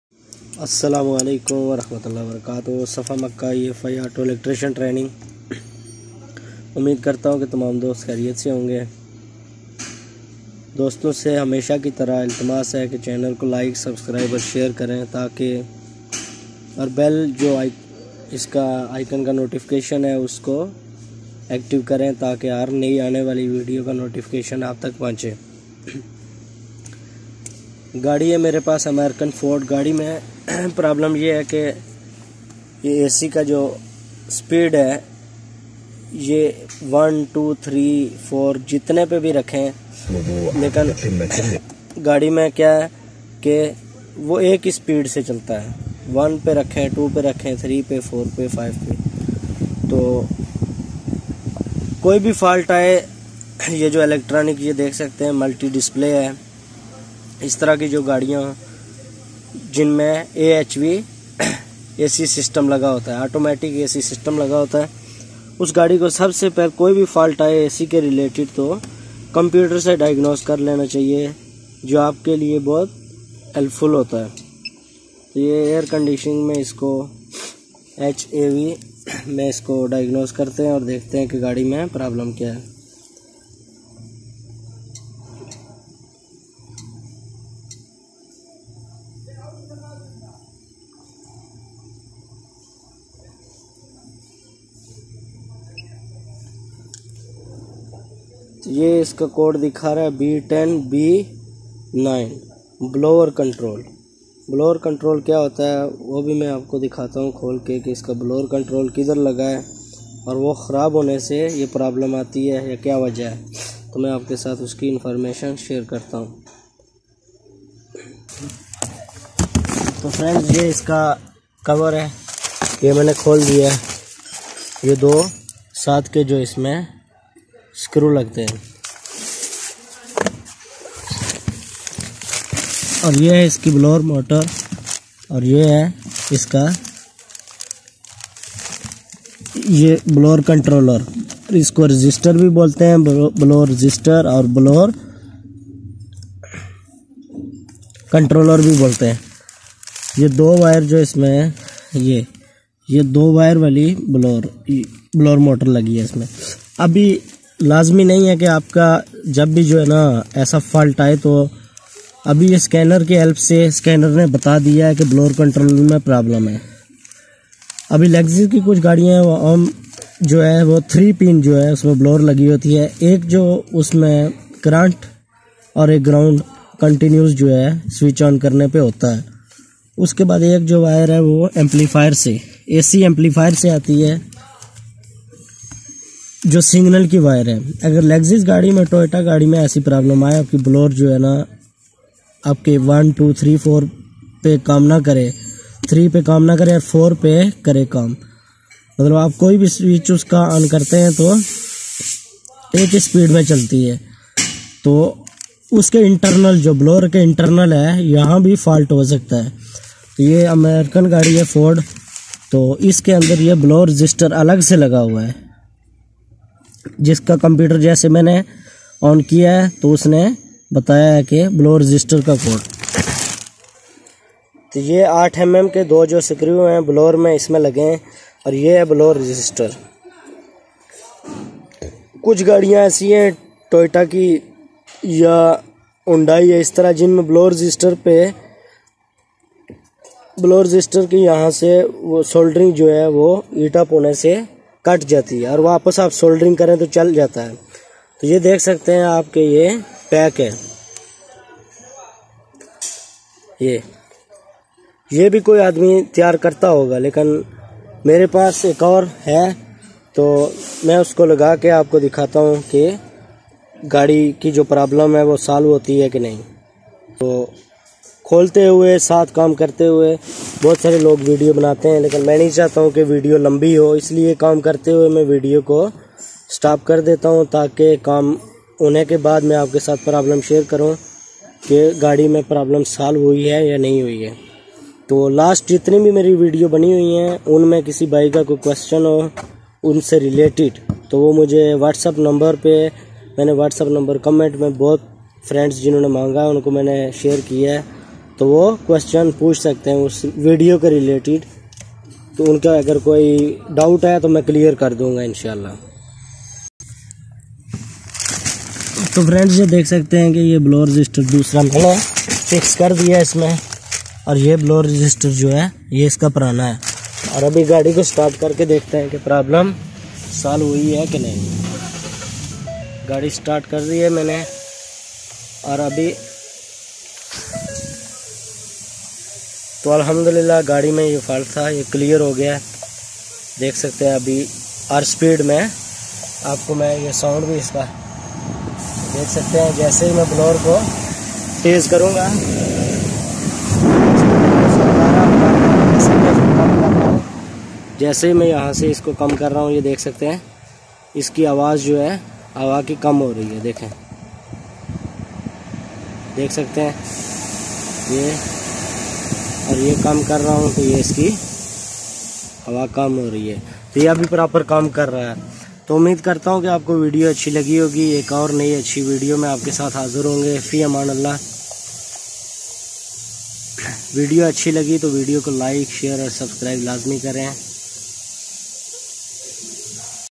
How we can Fix easily B10B9 in Ford Explorer 2013 14.Both other Vehicles have same problem. watch🚗🚗
WHAT IS B10B9 in Ford.
How we can blower speed problm in Ford.
What is blower control in Ford.